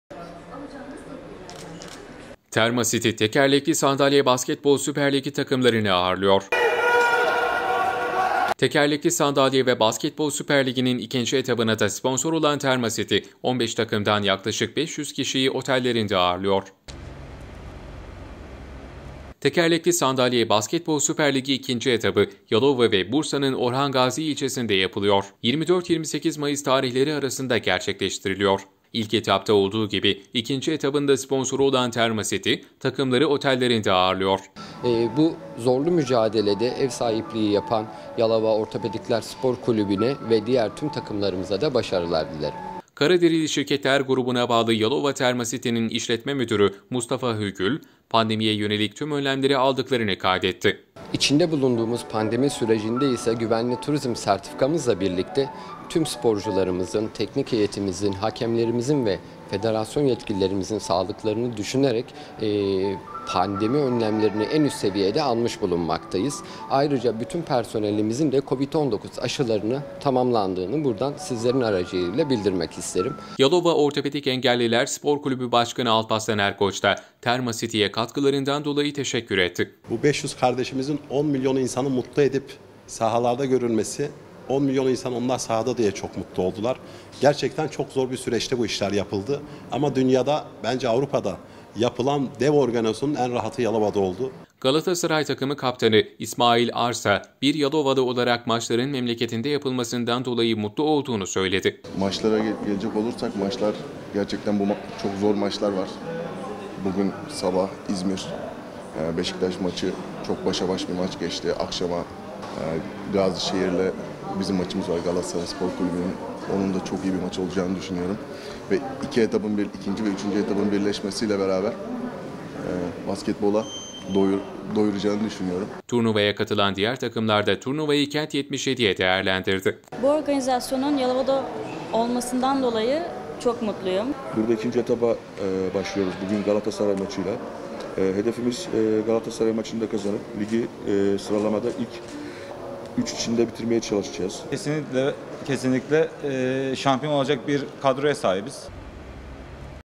TERMACİTY DEV ORGANİZASYONUN EV SAHİBİ
Tekerlekli Sandalye Basketbol Süper Ligi'nin ikinci etabına da sponsor olan Terma City, 15 takımdan yaklaşık 500 kişiyi otellerinde ağırlıyor.
Tekerlekli Sandalye Basketbol Süper Ligi ikinci etabı Yalova ve Bursa’nın Orhangazi ilçesinde yapılıyor. 24-28 Mayıs tarihleri arasında gerçekleştiriliyor.  İlk etapta olduğu gibi ikinci etabın da sponsoru olan Terma City, takımları otellerinde ağırlıyor.
Karaderili Şirketler Grubu’na bağlı Yalova Terma City'nin İşletme Müdürü Mustafa Hügül, pandemi yönelik tüm önlemleri aldıklarını kaydetti.
Yalova Ortopedik Engelliler Alpaslan Erkoç da Terma City’e katkılarından dolayı teşekkür etti.
Galatasaray takımı kaptanı İsmail Ar ise bir Yalovalı olarak maçların memleketinde yapılmasından dolayı mutlu olduğunu söyledi.
Turnuvaya katılan diğer takımlar da turnuvayı Kent77’ye değerlendirdi.